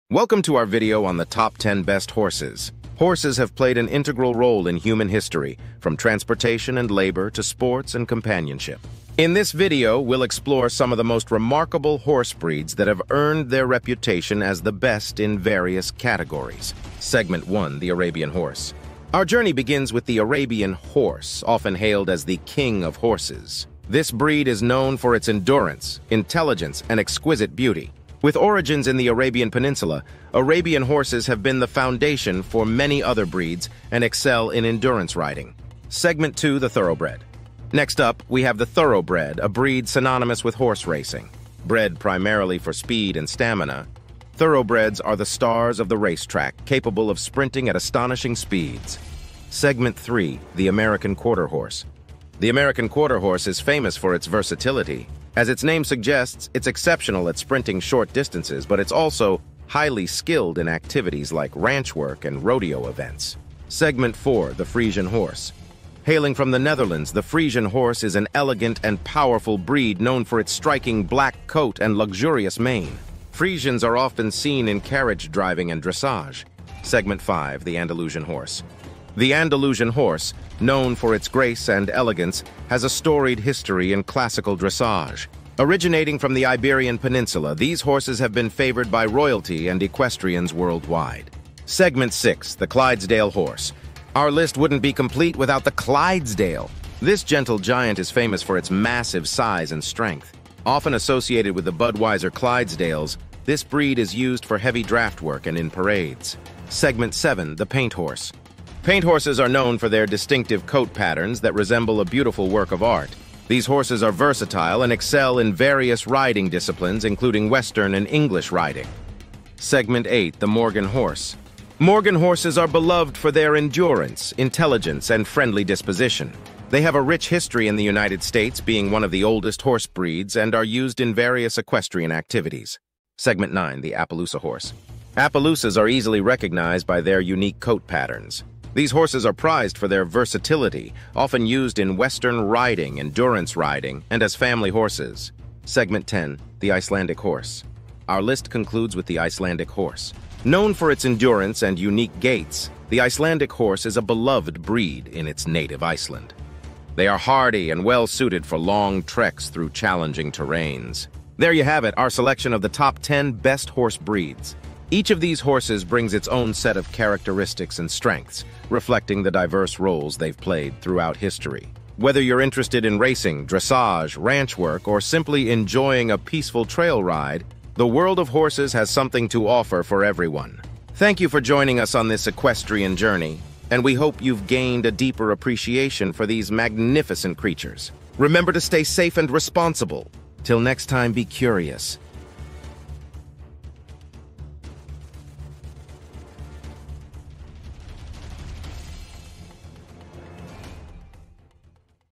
Top 10 Best Horses
Discover the world's "Top 10 Best Horses" 🏇🌟. From Arabian elegance to Thoroughbred speed, explore the unique qualities of these majestic animals! 🐴🌎

📝 Chapters:

0:00 Introduction.
0:00 [SEGMENT 1: The Arabian Horse].
0:00 [SEGMENT 2: The Thoroughbred].
0:00 [SEGMENT 3: The American Quarter Horse].
0:00 [SEGMENT 4: The Friesian Horse].
0:00 [SEGMENT 5: The Andalusian Horse].
0:00 [SEGMENT 6: The Clydesdale Horse].
0:00 [SEGMENT 7: The Paint Horse].
0:00 [SEGMENT 8: The Morgan Horse].
0:00 [SEGMENT 9: The Appaloosa Horse].
0:00 [SEGMENT 10: The Icelandic Horse].
0:00 Conclusion.
0:00 Outro.

Thanks.